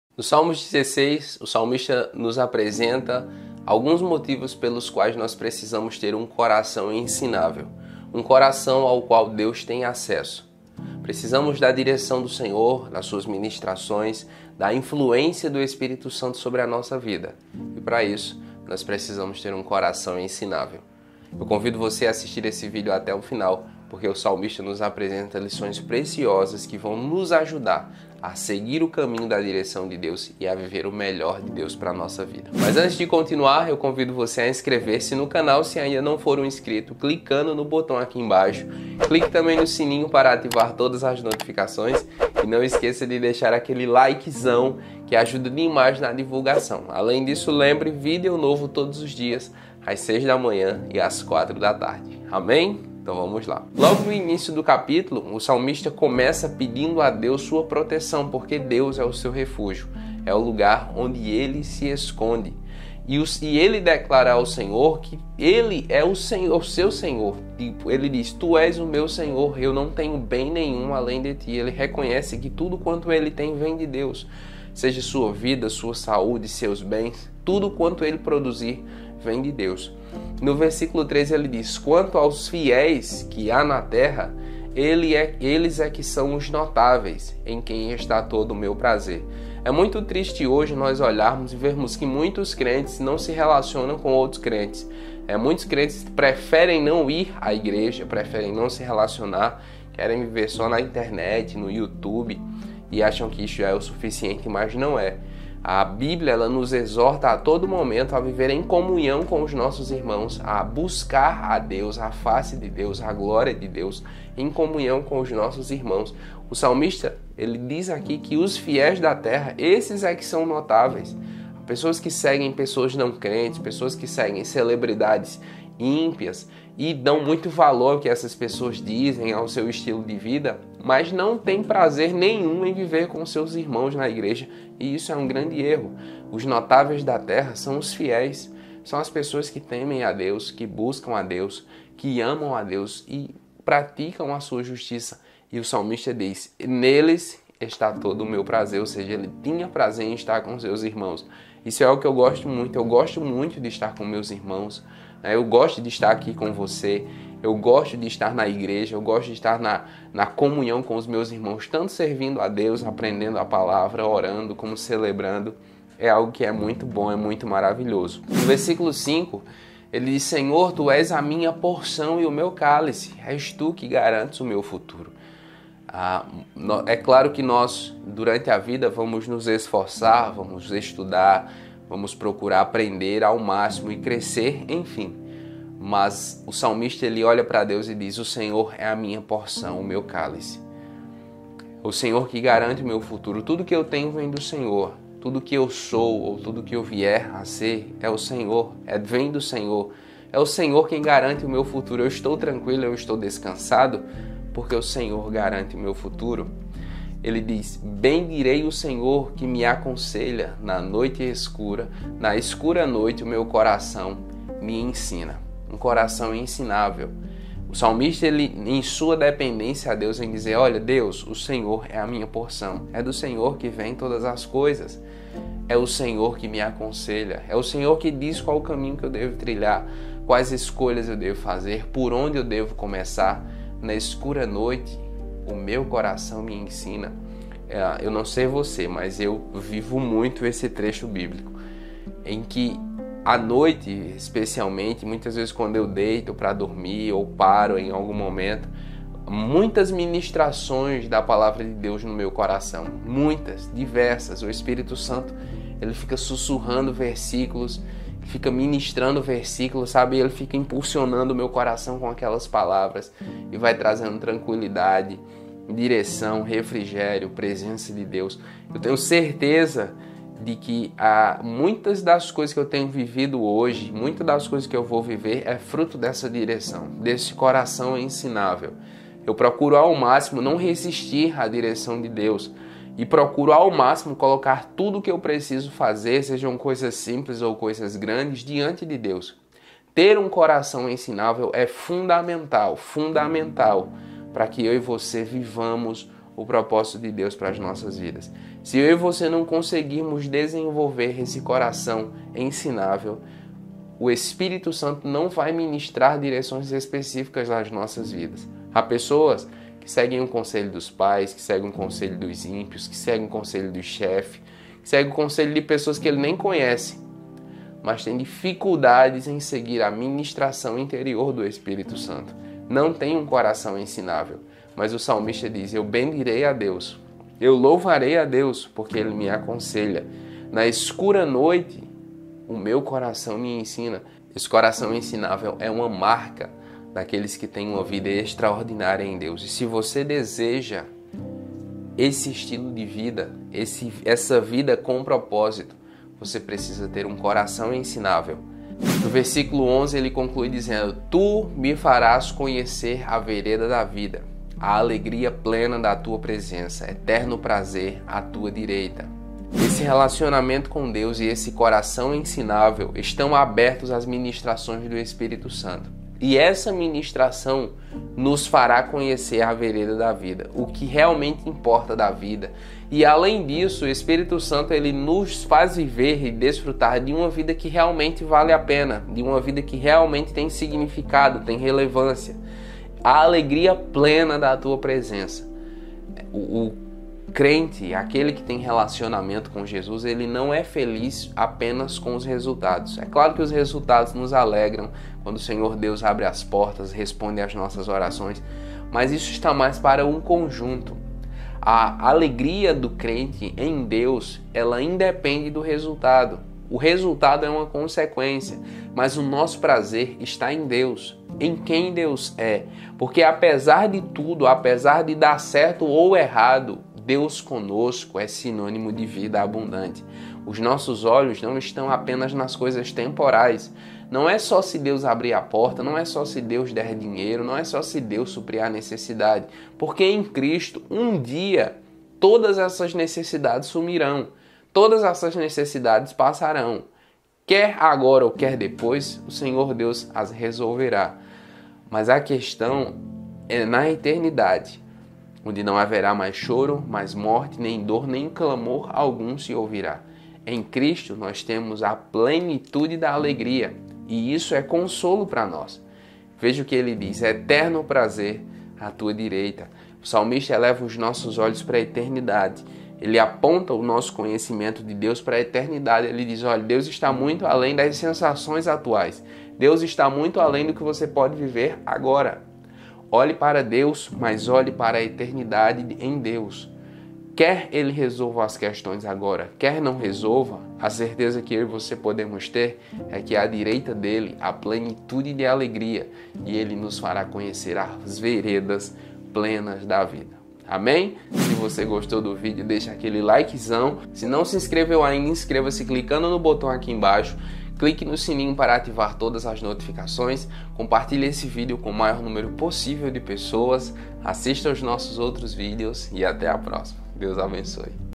Salmo 16 Estudo: Coração ENSINÁVEL (Bíblia Explicada)
No Salmos 16, o salmista faz uma verdadeira declaração de amor ao Senhor Deus. Ele diz: “Tu és o meu Senhor; não tenho bem nenhum além de ti”. Deus é o seu maior bem. Não há nada nem ninguém que dispute a alegria de ter o Senhor por perto. Davi o ama, e isso é inegociável.